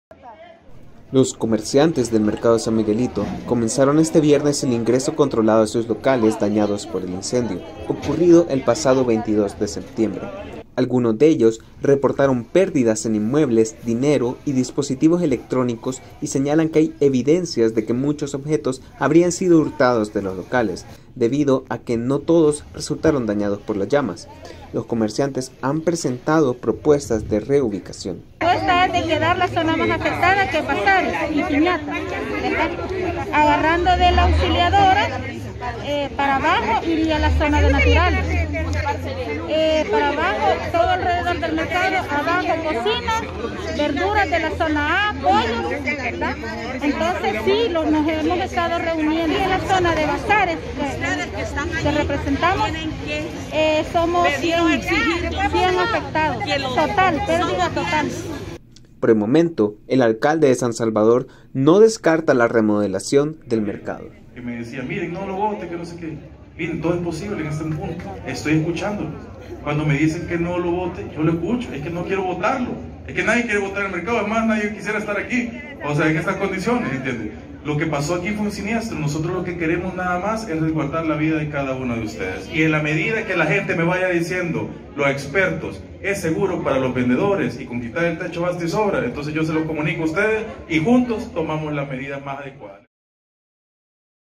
Comerciantes del mercado San Miguelito rescatan lo poco que el incendio no calcinó
Los comerciantes del mercado San Miguelito comenzaron este viernes el ingreso controlado a sus locales dañados por el incendio, ocurrido el pasado miércoles 22 de septiembre.

¡Visítenos!